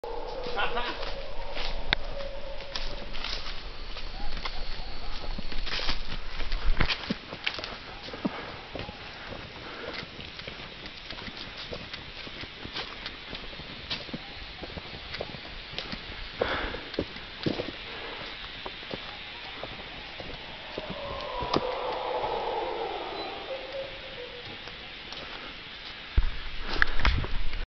Corcovado NP Costa Rica
This is a movie from the Corcovado NP in Costa Rica,..were we spend 1 week in the forest with Howler Monkeys, Red parrots, Crocodiles,..and many more!